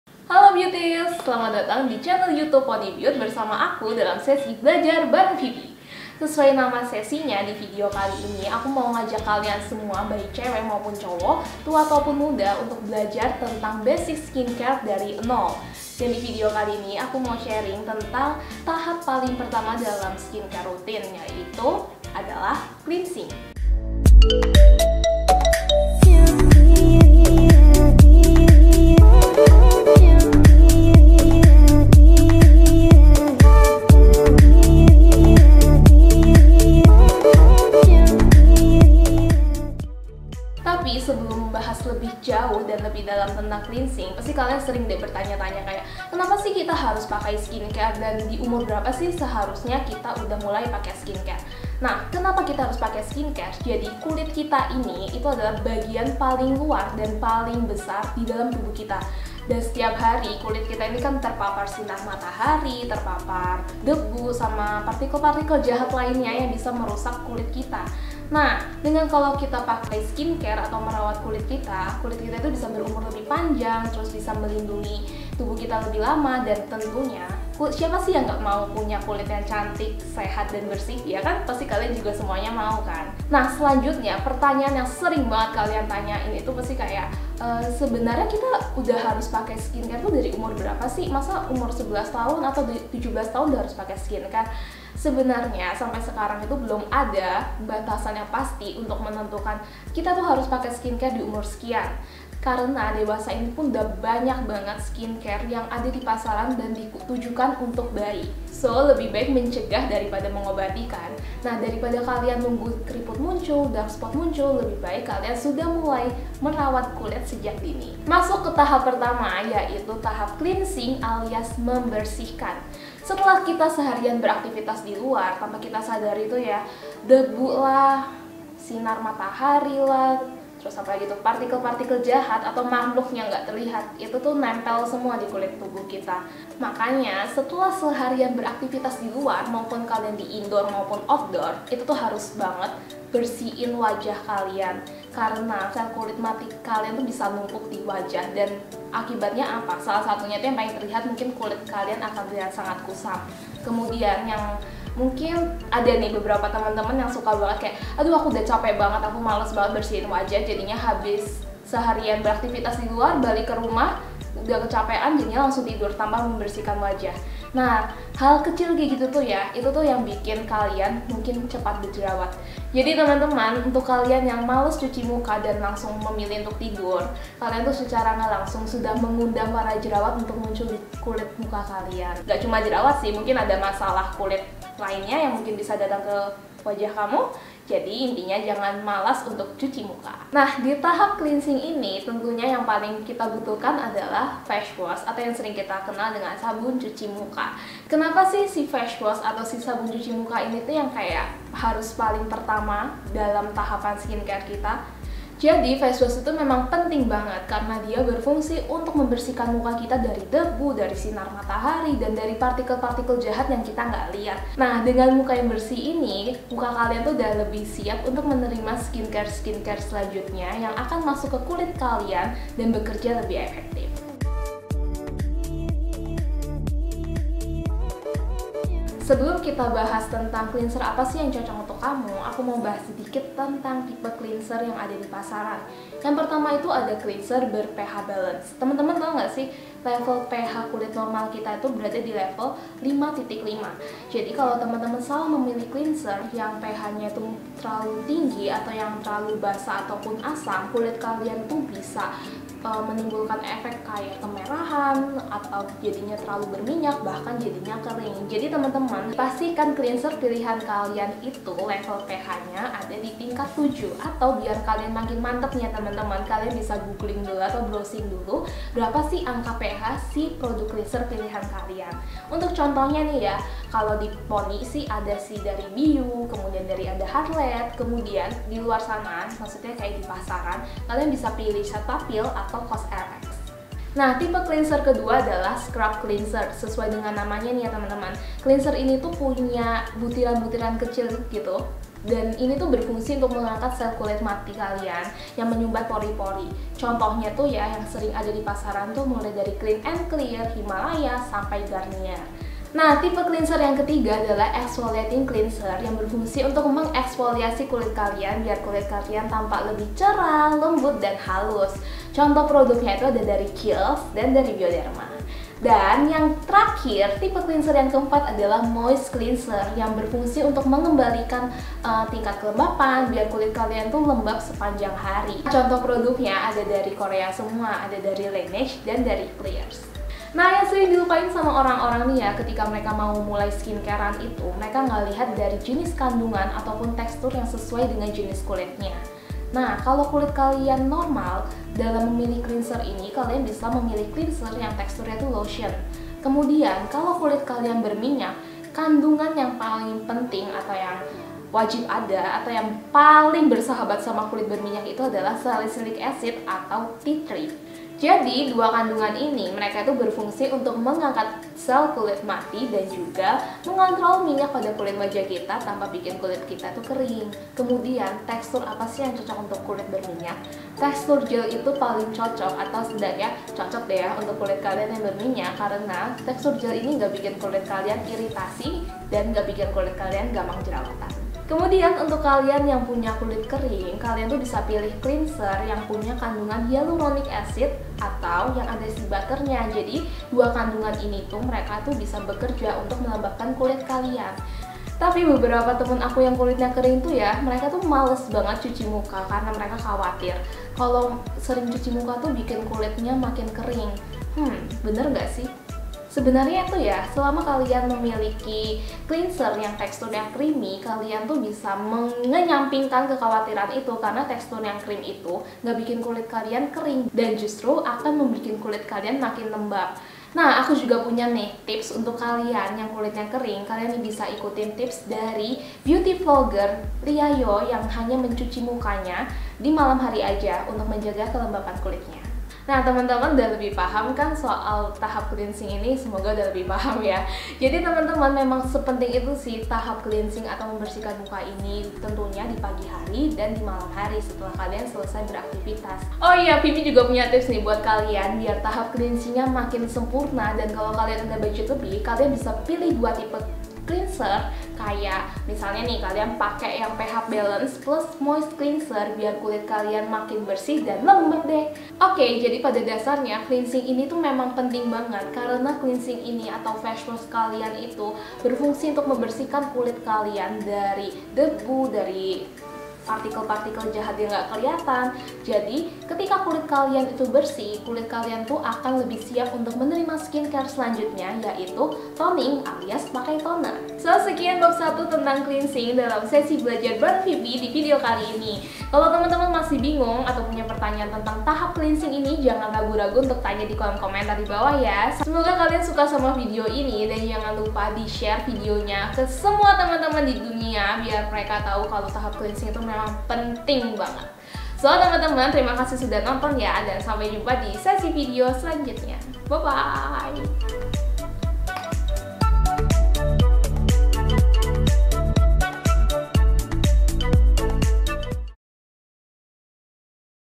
[Bab 1] Cara Pilih Cleanser untuk Pemula | Belajar Bareng Phoebe : Basic Skincare Routine
Hello Phoebe's,

A new fresh start in Ponny Beaute
Siapa nih yang masih suka bingung sama skincare routine atau baru mau mulai pakai skincare?
Biar nggak salah langkah, kamu bisa tonton video baru dari Ponny Beaute dalam series: Belajar Bareng Phoebe!
Dalam video bab 1 ini, Phoebe akan bahas seputar tahap pertama skincare yaitu cleansing. Kamu bakal tau cara pilih cleanser sesuai jenis kulit biar nggak timbul masalah kulit. Langsung tonton video selengkapnya di YouTube Ponny Beaute ya!

xoxo

Line :: @ponnybeaute